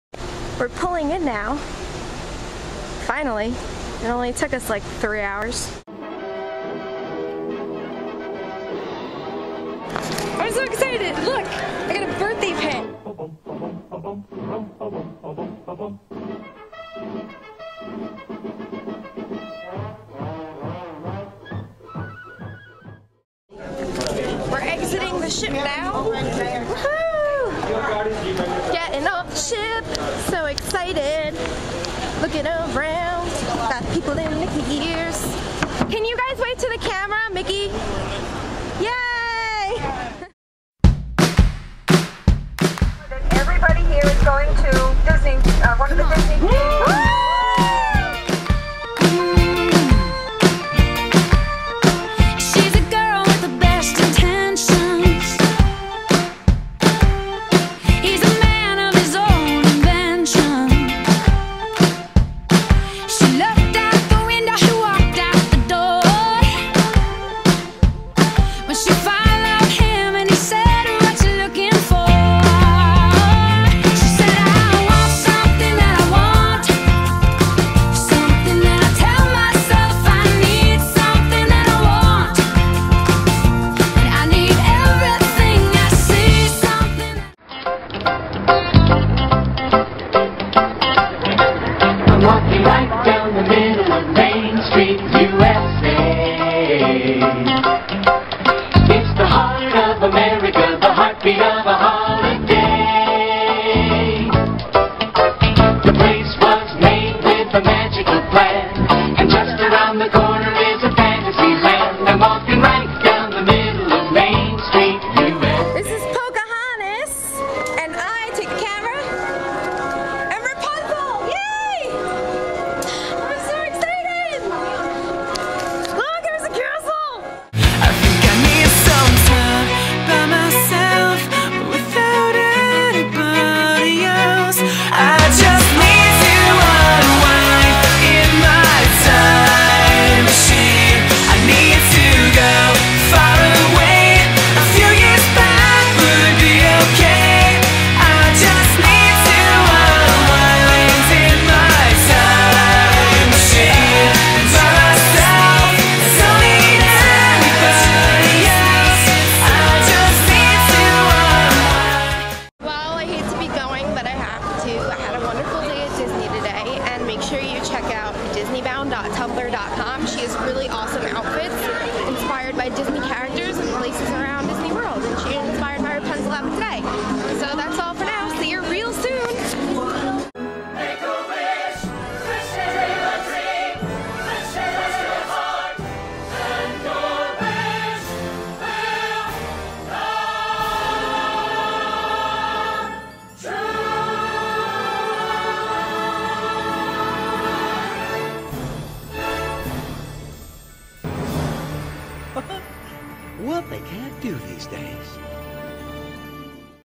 Disney Diaries Episode 1: A Day in The Magic Kingdom
I thought I'd capture the memories of my most recent Disney Trip!

I own: All footage and pictures of the theme park!

I do not own: Any music, the DisneyBound outfits (they are uploaded with permission by Leslie), the picture of Jiminy Cricket.